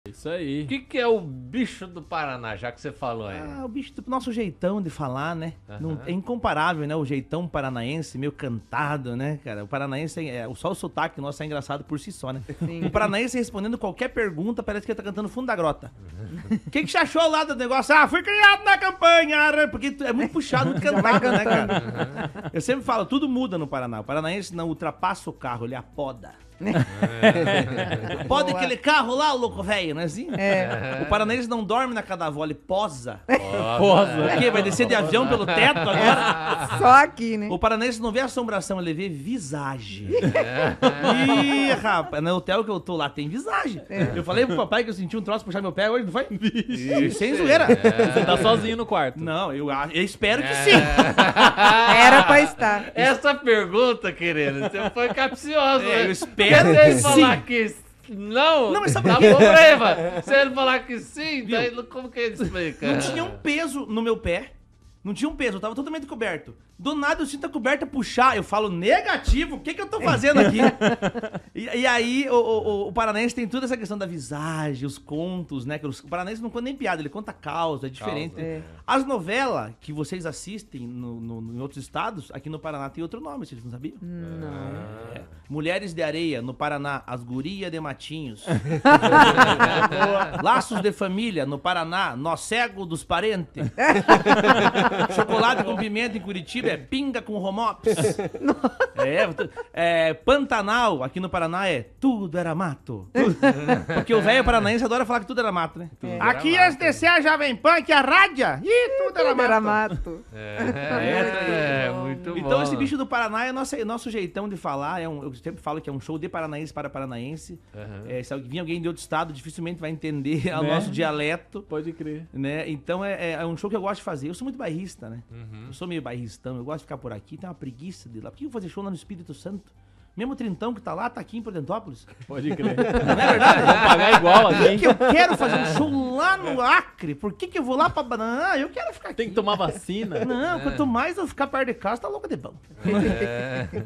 ALORINO JR. CONTA POR QUE O PARANAENSE É DIFERENCIADO
Veja ao RCC News de segunda a sexta às 7h da manhã. Conheça o Parlatório às 18h.